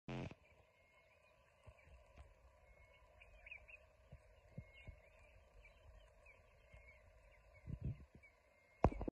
Zebra tail wagging contest!
A group of zebras drink water at a waterhole close to Punda Maria Rest camp in the Kruger National Park. Video by M. de Castro.